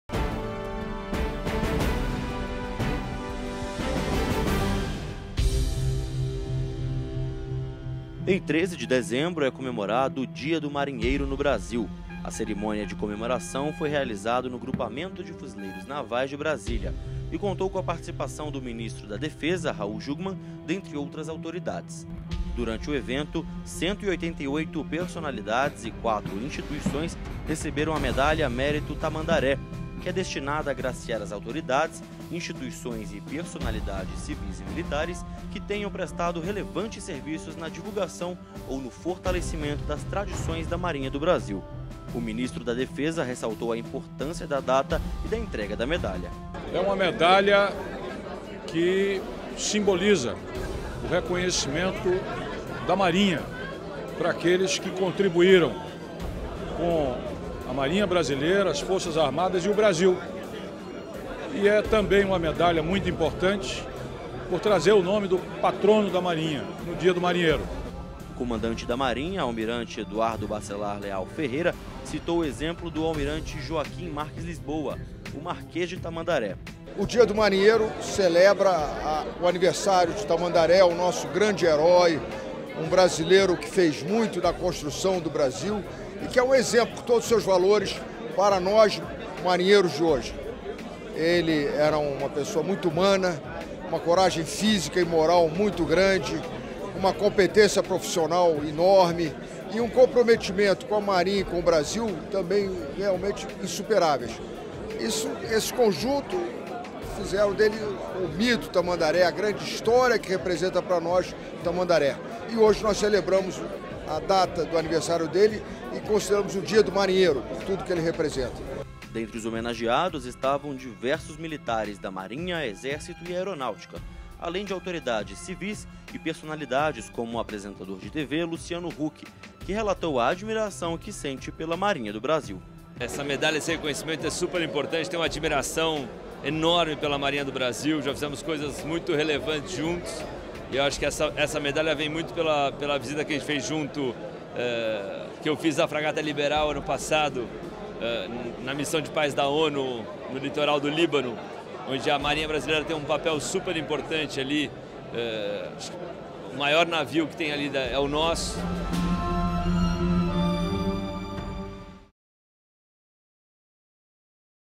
TV Defesa: Solenidade e entrega de medalhas marcam o Dia do Marinheiro
Durante cerimônia de comemoração ao Dia do Marinheiro, em Brasília, 188 personalidades e quatro instituições receberam a Medalha Mérito Tamandaré, que é destinada a agraciar as autoridades, instituições e personalidades civis e militares que tenham prestado relevantes serviços na divulgação ou no fortalecimento das tradições da Marinha do Brasil.